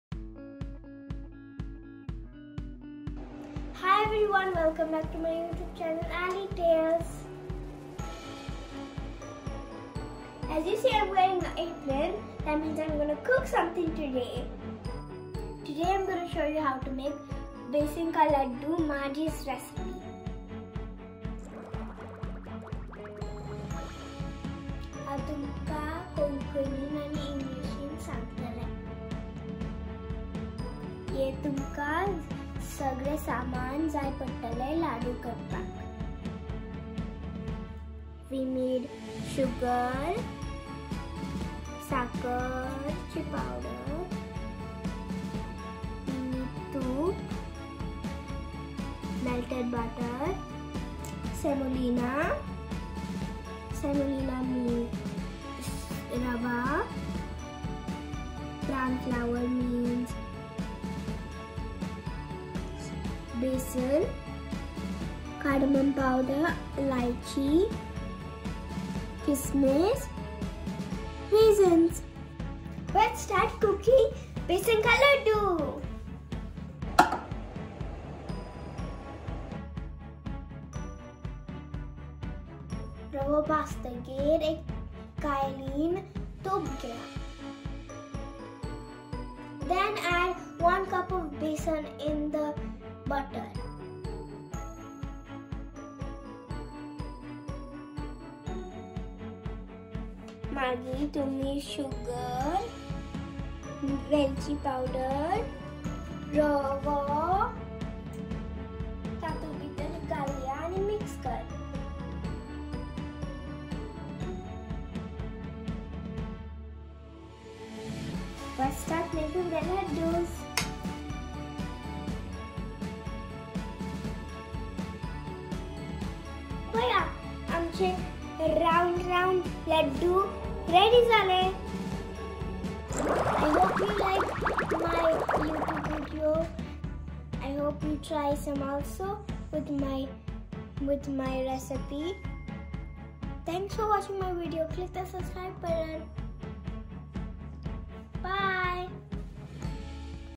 AleeTales Episode 5 - Maa Aaji special besan rava ladoos
This time i am trying to cook my grand mom's (Maa Aaji's) recipe ... "Besan Rava Ladoo" .. these are sweet balls and i am sure you wil love it...please do try it with your parents .. like/Share/Subscribe .. loadsa love
This is also my attempt to speak and do a video in Konkai :-) along with English offcourse.